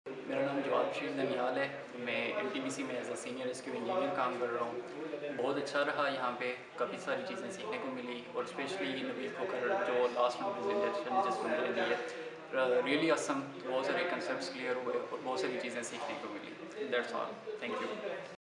Jawad Bashir: Testers Meetup Organized by Code Movement Pakistan
Jawad Bashir is sharing his thoughts about Testers meetup arranged by Code Movement Pakistan on 21st of August, 2016.